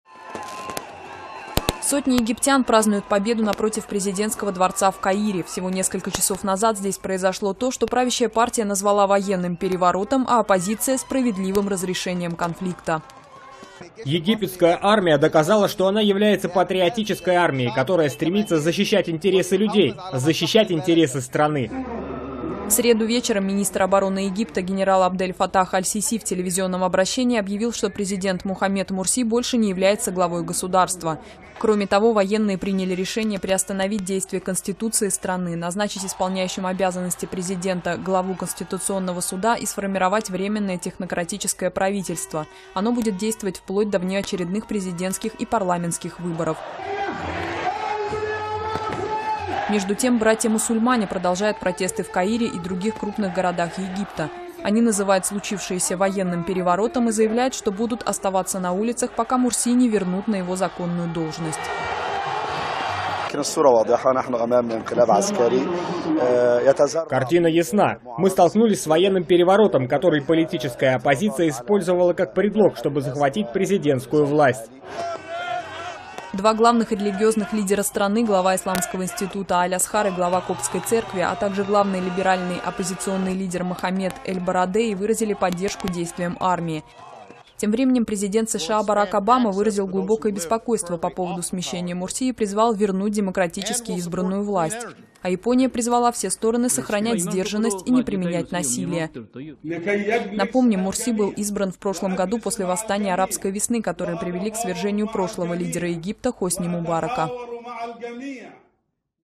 Президента Египта отстранили от власти (новости)
Президента Египта отстранили от власти. Президента Египта, исламиста Мухаммеда Мурси, в среду отстранили от власти. Об этом в телевизионном обращении объявил глава армии страны.

Сотни египтян празднуют победу напротив президентского дворца в Каире. Всего несколько часов назад здесь произошло то, что правящая партия назвала военным переворотом, а оппозиция -- справедливым разрешением конфликта.

[Фарук Дарвиш, протестующий]:
«Египетская армия доказала, что она является патриотической армией, которая стремится защищать интересы людей, защищать интересы страны».

В среду вечером министр обороны Египта генерал Абдель Фаттах аль-Сиси в телевизионном обращении объявил, что президент Мухаммед Мурси больше не является главой государства.

Кроме того, военные приняли решение приостановить действие Конституции страны, назначить исполняющим обязанности президента главу Конституционного суда и сформировать временное технократическое правительство. Оно будет действовать вплоть до внеочередных президентских и парламентских выборов.

Между тем, «Братья-мусульмане» продолжают протесты в Каире и других крупных городах Египта. Они называют случившееся военным переворотом и заявляют, что будут оставаться на улицах, пока Мурси не вернут на его законную должность.

[Мухаммед аль-Бальтаги, лидер «Братьев-мусульман»]:
«Картина ясна: мы столкнулись с военным переворотом, который политическая оппозиция использовала как предлог, чтобы захватить президентскую власть».

Два главных религиозных лидера страны, глава Исламского института Аль-Азхар и глава Коптской церкви, а также главный либеральный оппозиционный лидер Мохаммед эль-Барадеи выразили поддержку действиям армии.

Тем временем, президент США Барак Обама выразил глубокое беспокойство по поводу смещения Мурси и призвал вернуть демократически избранную власть. А Япония призвала все стороны сохранять сдержанность и не применять насилие.

Напомним, Мурси был избран в прошлом году после восстаний Арабской весны, которые привели к свержению прошлого лидера Египта Хосни Мубарака.